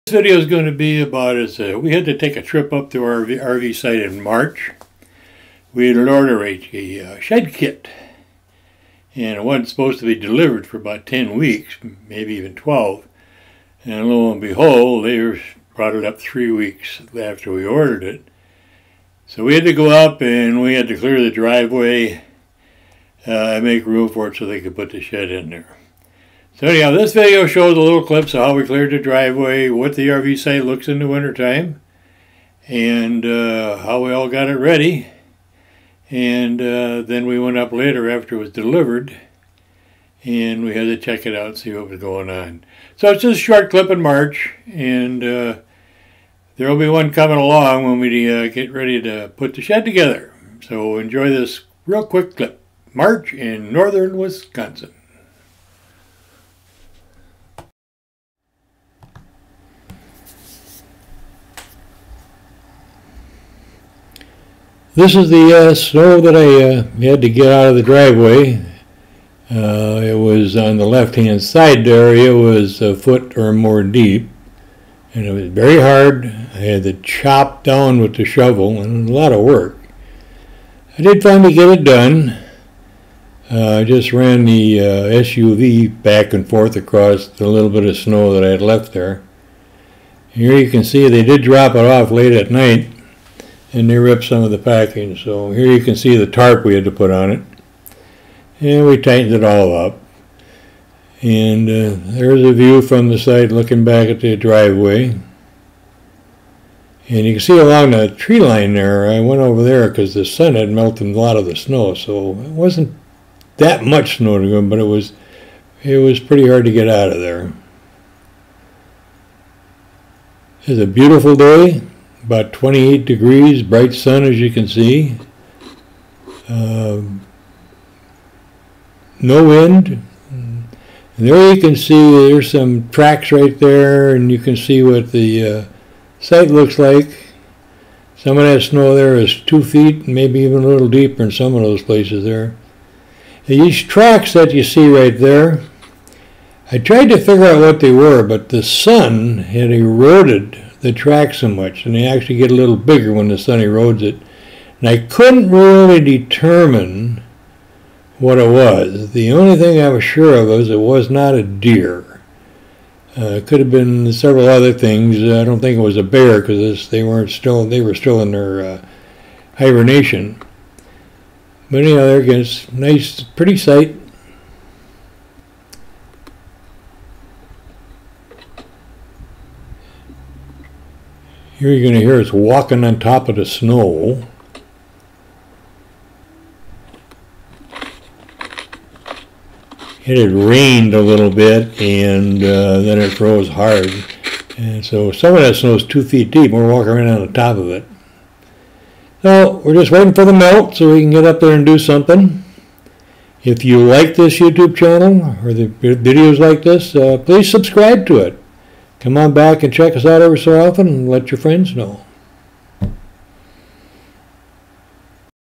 March 2022 shed delivery 2
We ordered a shed kit for our RV Site in northern Wisconsin. The manufacturer delivered it early and we had to go and clear a path through the snow. It also shows our permanent RV site in the wintertime. Lots of snow and wildlife tracks.